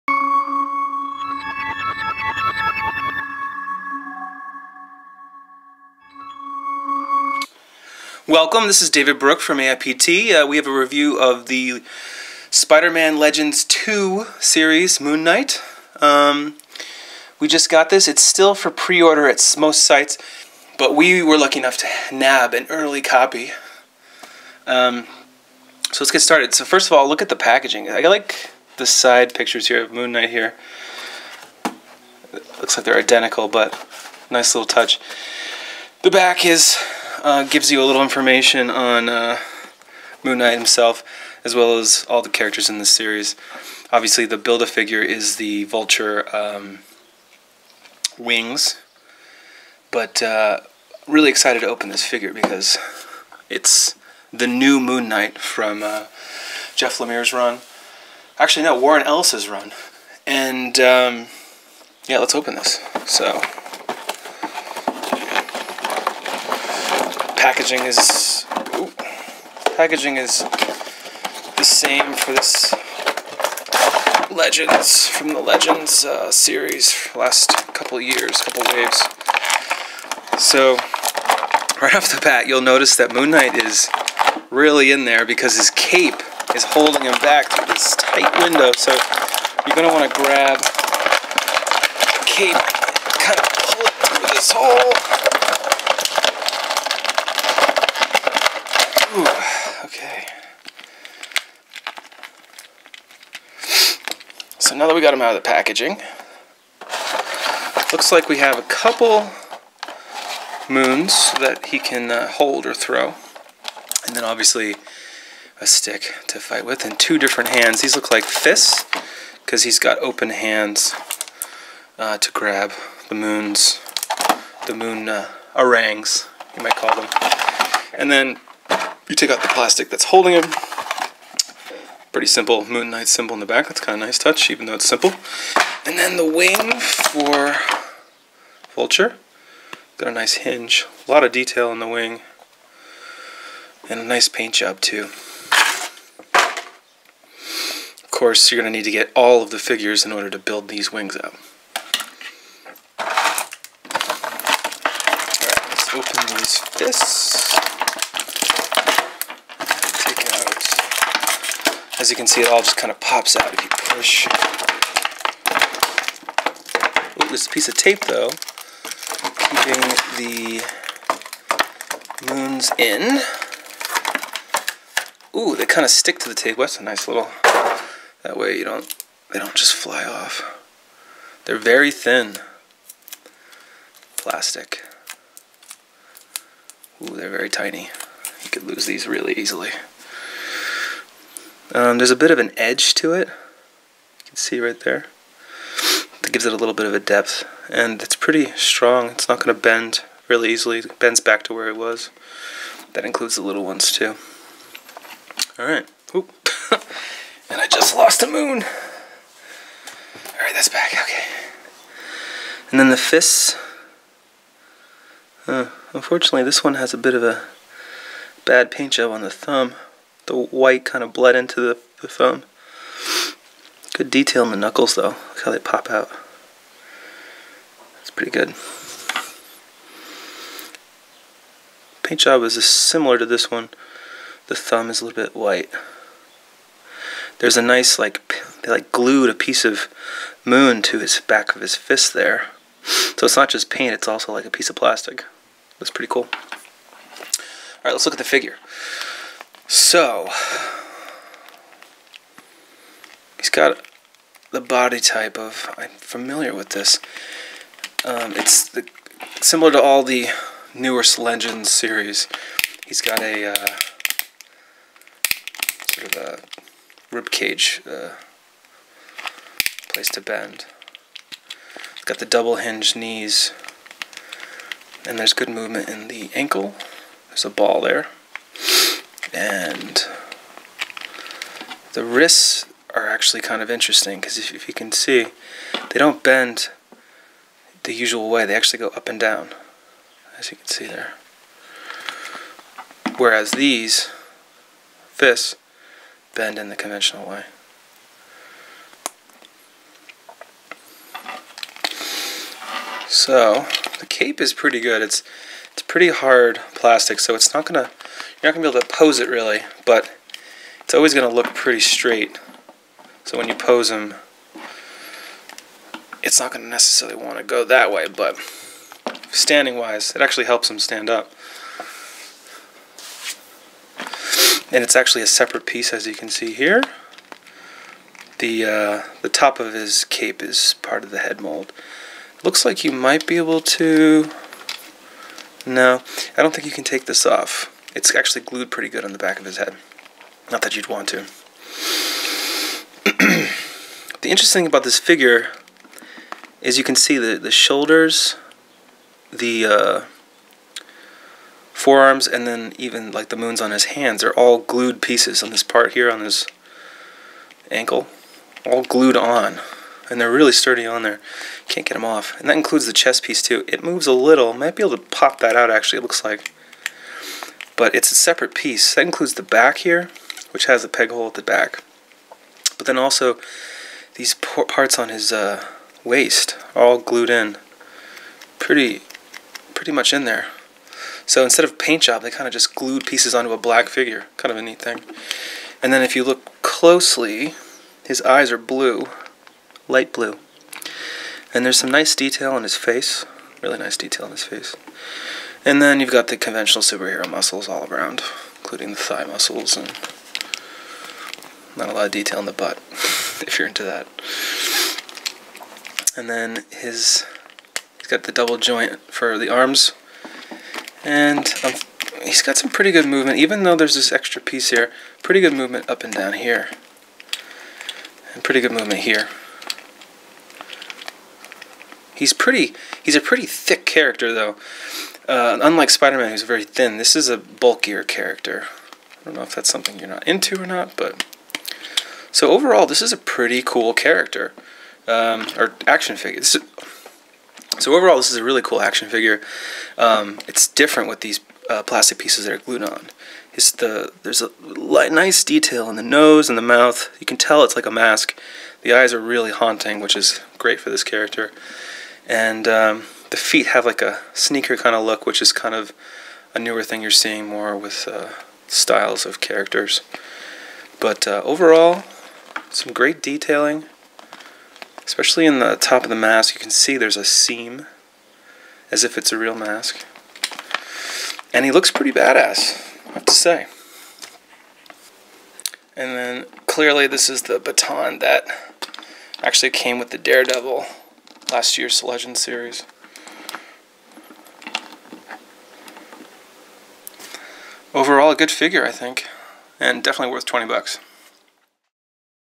Unboxing/Review: Marvel Legends 6" Modern Moon Knight Action Figure
This is a unboxing and review of the recent Marvel Legends The Amazing Spider-Man 2 Infinite series Moon Knight. This is one of 7 characters each with a build a figure piece to create Vulture's wings.